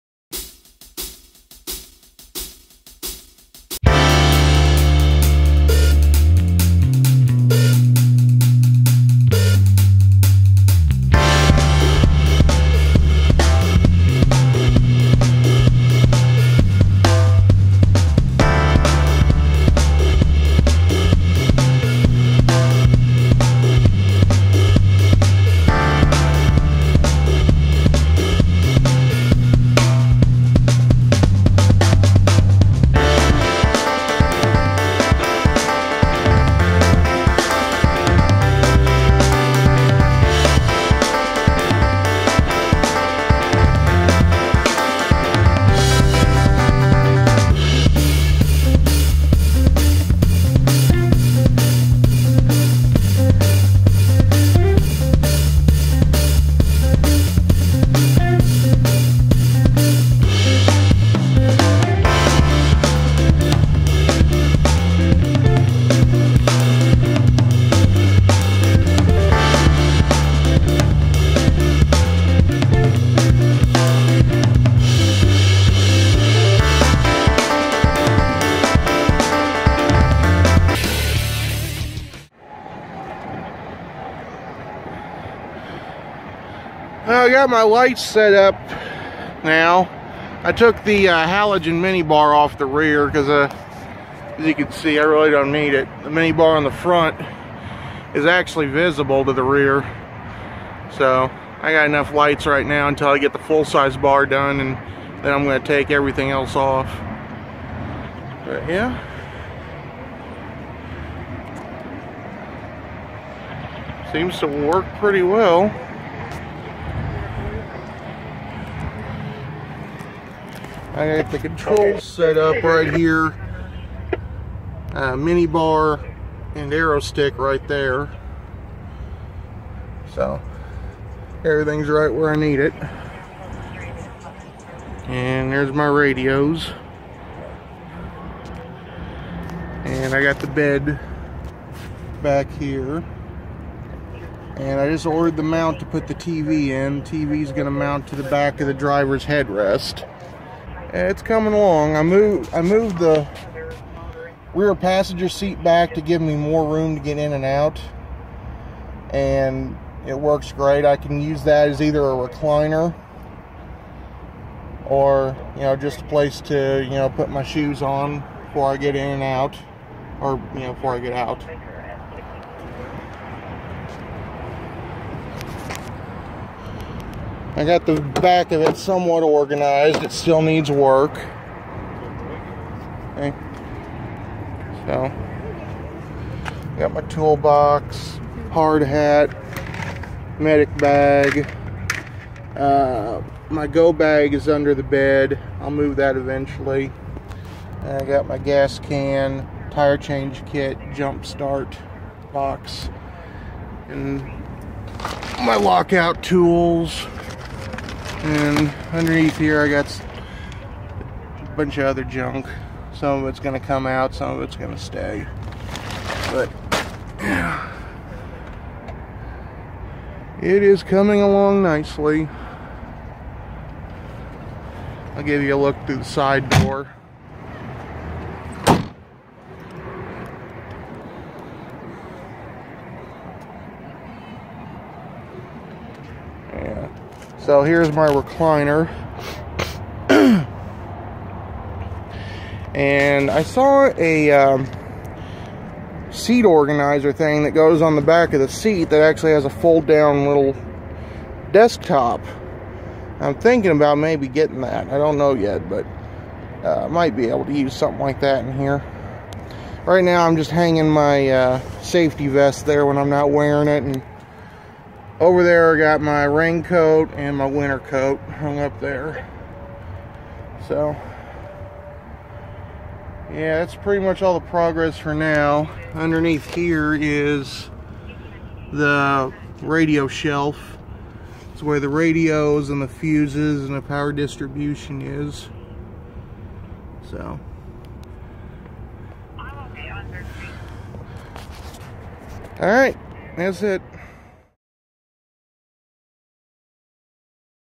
My 98 Astro Roadside Service/Search and Rescue Van
My new vehicle to replace my wrecked truck. It's equipped for my work in roadside assistance and volunteer work in search and rescue.